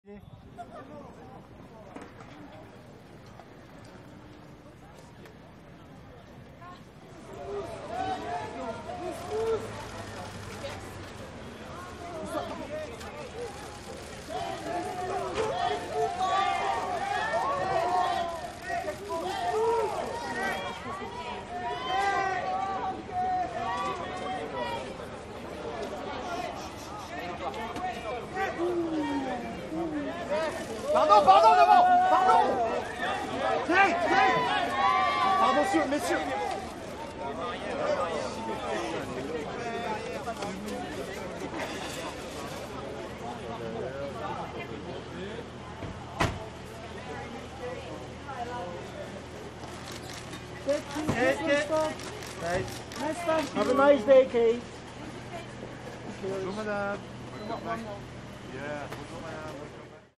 Kate Moss attending the Louis Vuitton Men Fashion Show in Paris
We spotted the Fashion icon Kate Moss swarmed by the fans and photographers at Louis Vuitton Men Fashion Show in Paris.

January 2015, Thursday 22nd, Paris, France